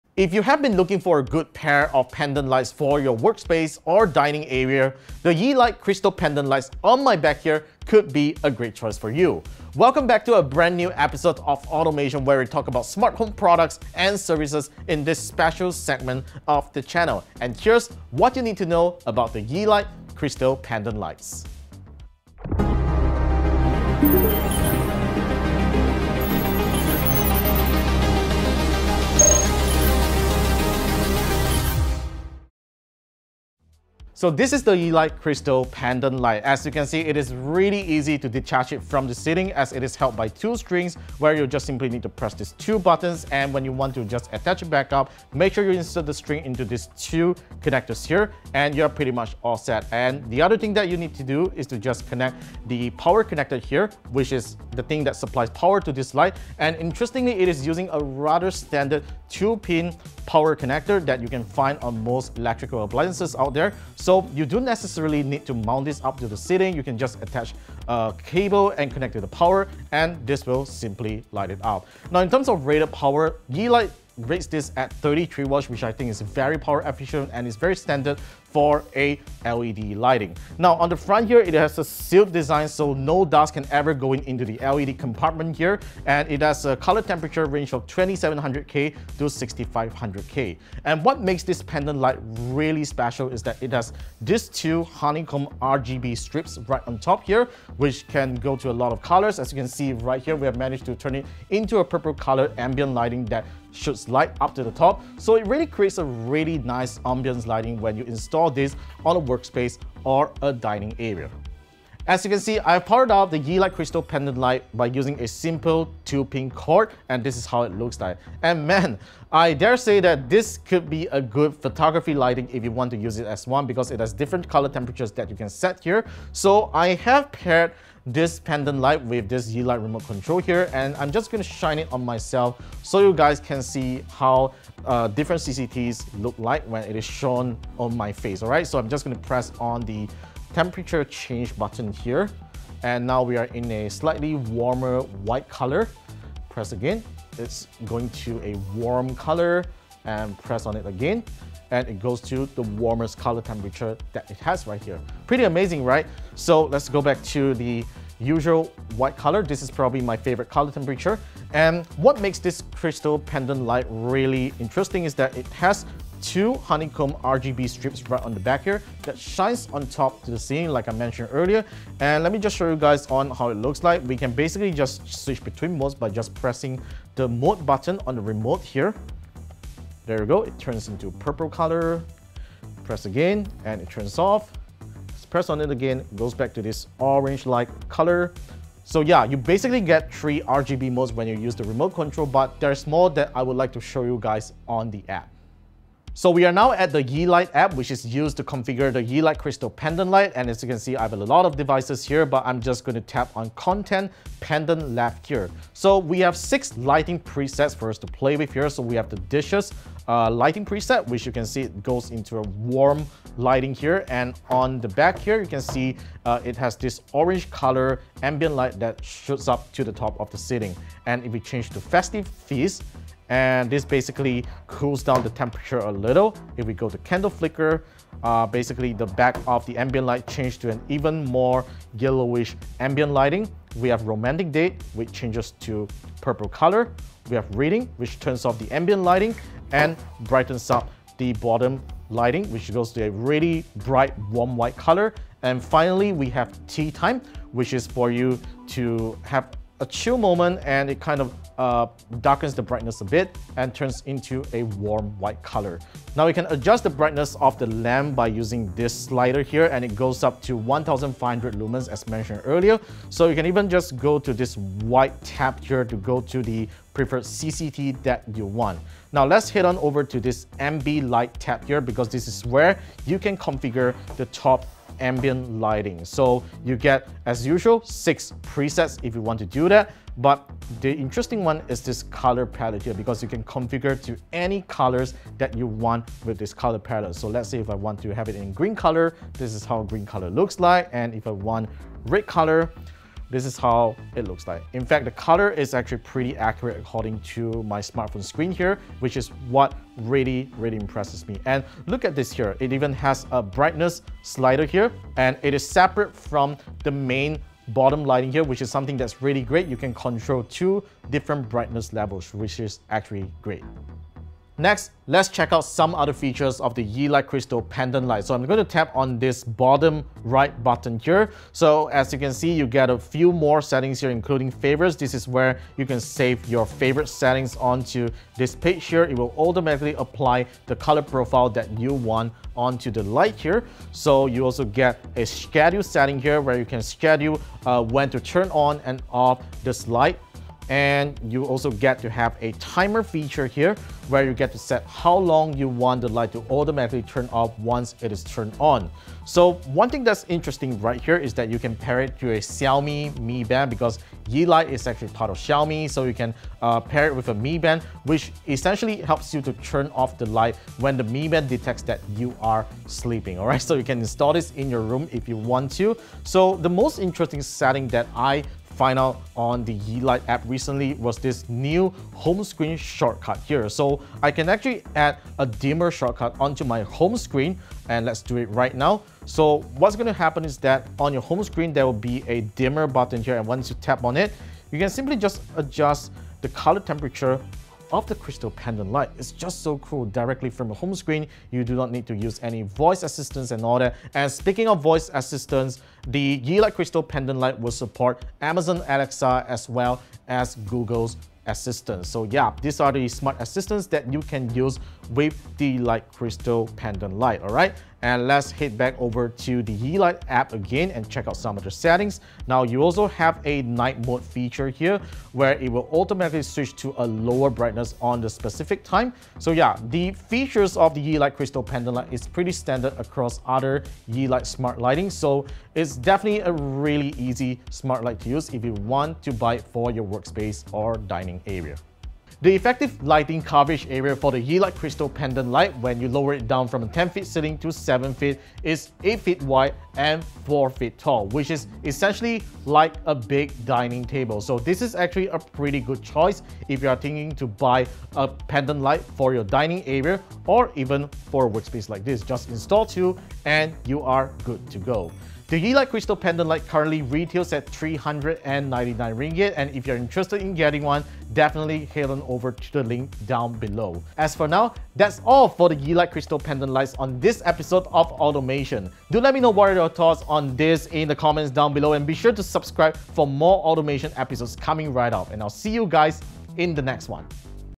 The Best Smart Pendant Light for Dining and Work | Automation Ep. 10
In this episode of Automation, we checked out the Yeelight Crystal Pendant Light and talked about its features, configuration and coverage area, it is very suitable for dining and workplaces due to its adjustable CCT and top ambient light. Best of all, it is really affordable at just RM549.